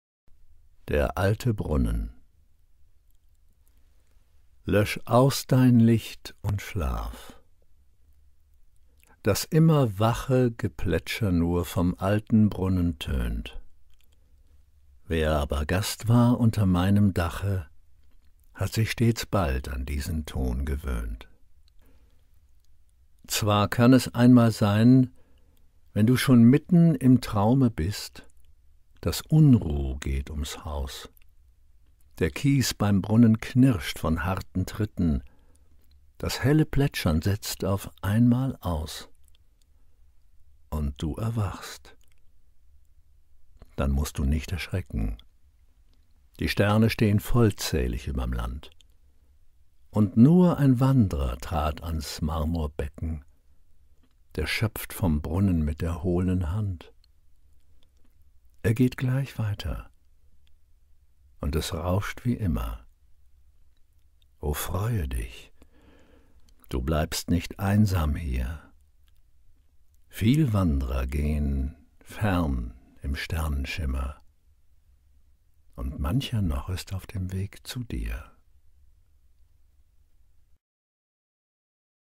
Hans Carossa „Der alte Brunnen"
Text:
Lösch aus dein Licht und schlaf! Das immer wache
Gesplätscher nur vom alten Brunnen tönt.
Wer aber Gast war unter meinem Dache,
Hat sich stets bald an diesen Ton gewöhnt.

Zwar kann es einmal sein, wenn du schon mitten
Im Traume bist, daß Unruh geht ums Haus,
Der Kies beim Brunnen knirscht von harten Tritten,
Das helle Plätschern setzt auf einmal aus,

Und du erwachst, — dann mußt du nicht erschrecken!
Die Sterne stehn vollzählig überm Land,
Und nur ein Wandrer trat ans Marmorbecken,
Der schöpft vom Brunnen mit der hohlen Hand.

Er geht gleich weiter. Und es rauscht wie immer.
O freue dich, du bleibst nicht einsam hier.
Viel Wandrer gehen fern im Sternenschimmer,
Und mancher noch ist auf dem Weg zu dir.

Bilder: Fotocollage